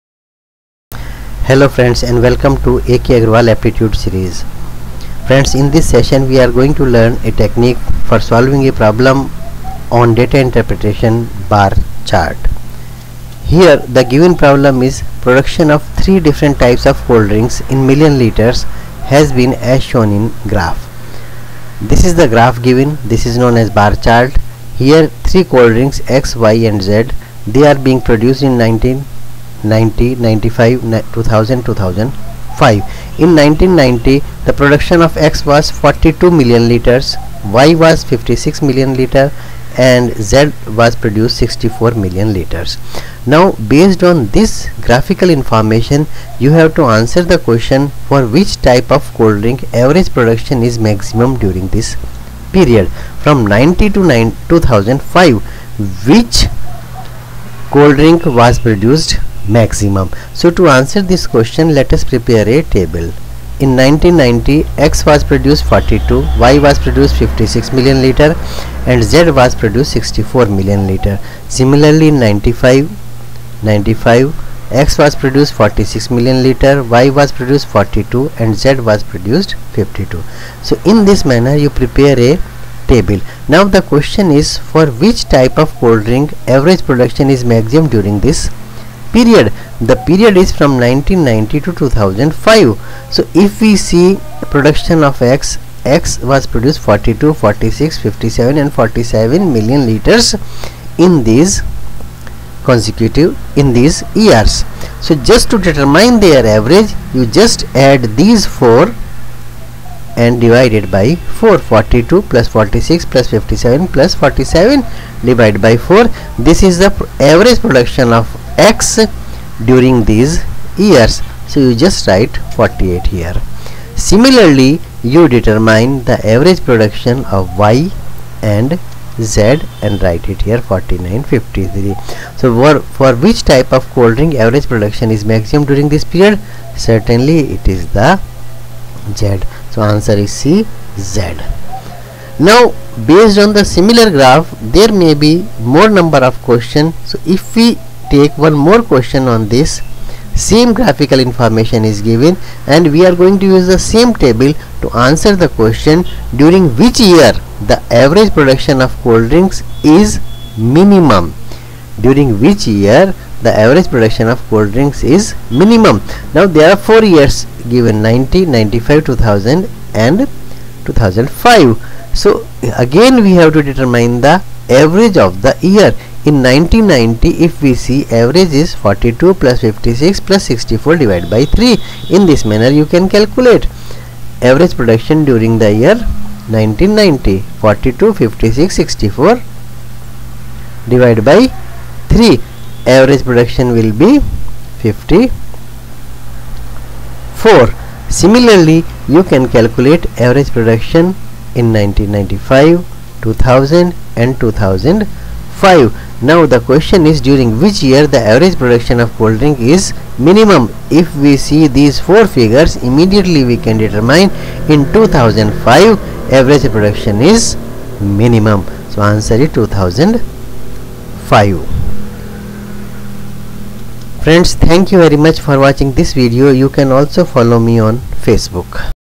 Data Interpretation Bar Chart, Problem 1,
It consists of a detailed description in a very simple format, for solving a problem based on bar chart.